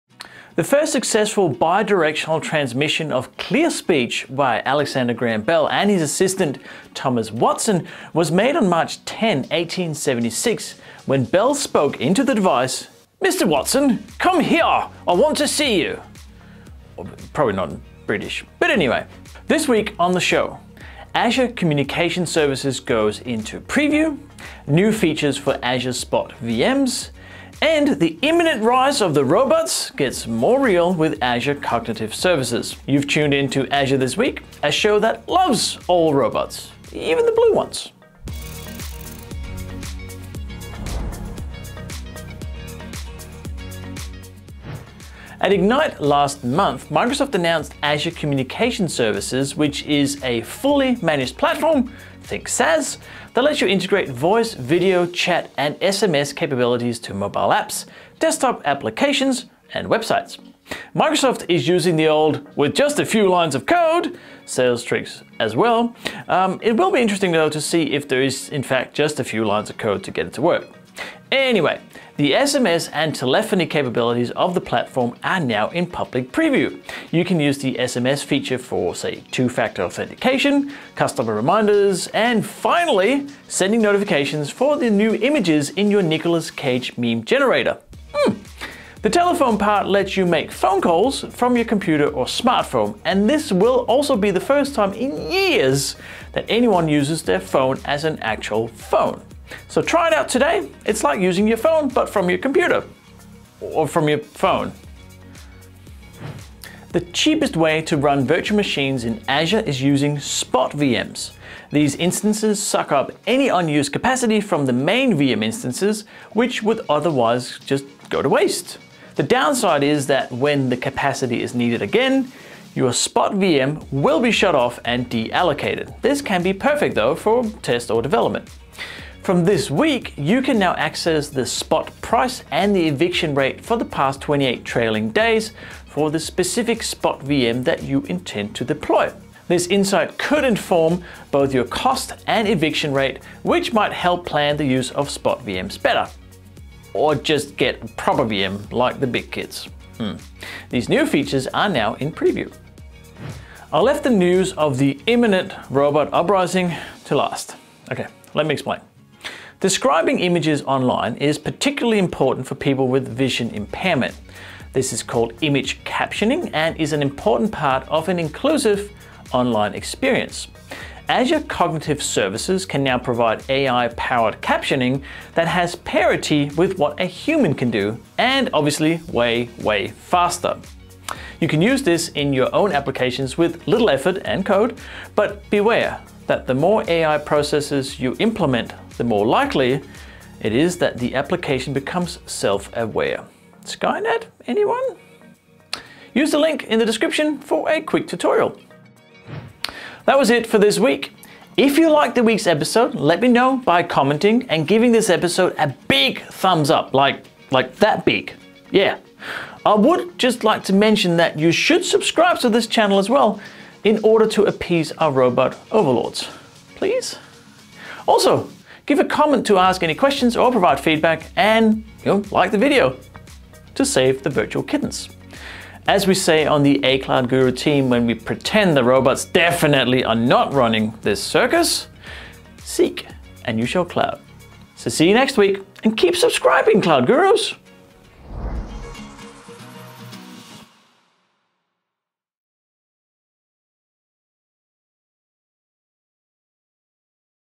Azure This Week - Azure Communication Services in Preview
This time in Azure news, Azure Communication Services goes into preview, new features for Azure Spot VMs, and the imminent rise of the robots gets more real with Azure Cognitive Services.